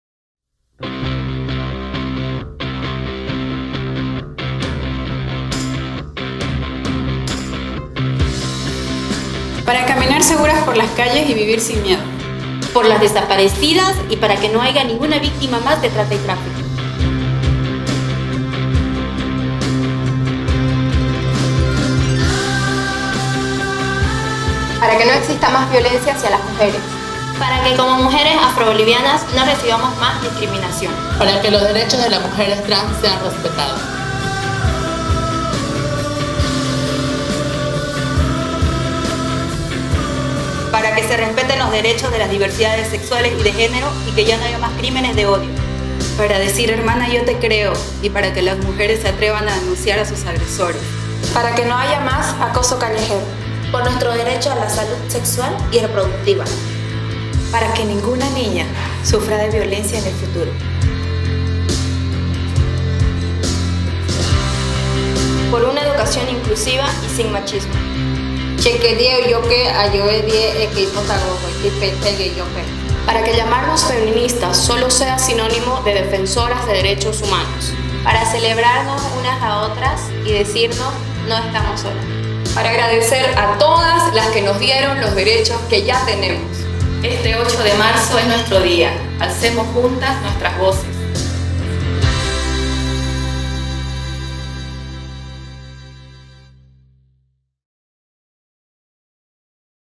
Convocatoria Santa Cruz 8M 2019
Convocatoria a la Marcha por el Dia Internacional de la Mujer Trabajadora - 8 de Marzo de 2019 para la ciudad de Santa Cruz Bolivia